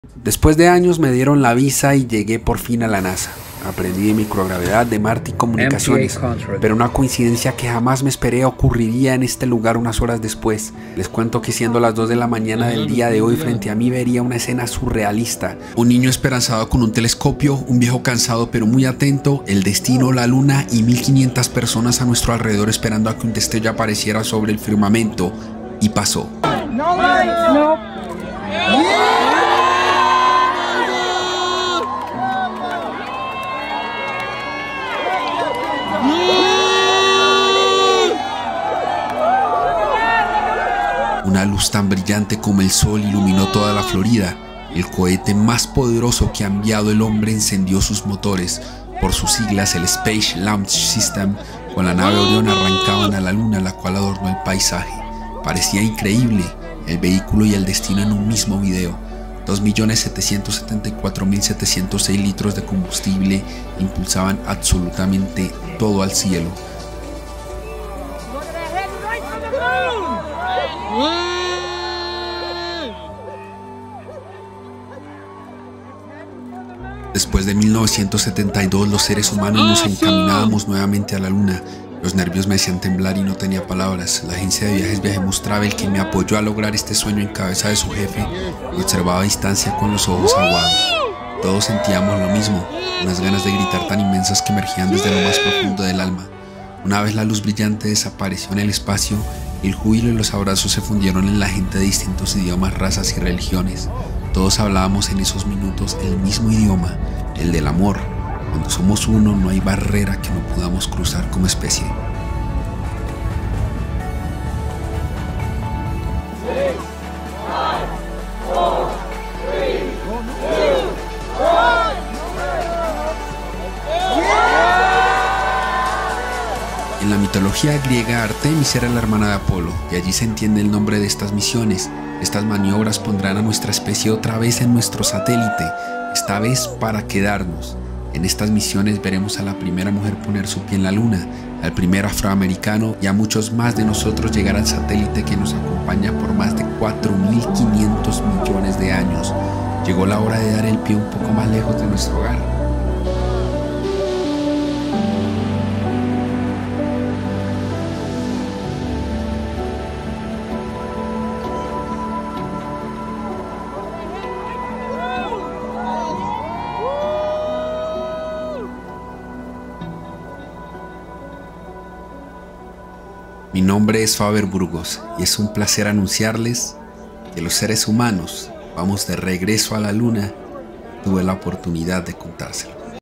Viaje a la NASA y lo que ví lo tienen que ver todos: El regreso del hombre a la Luna
Esto fue lo que viví hoy en las horas de la mañana.
Lo que ví no tiene precio.

Recuerden seguir a la mejor agencia de viajes del mundo.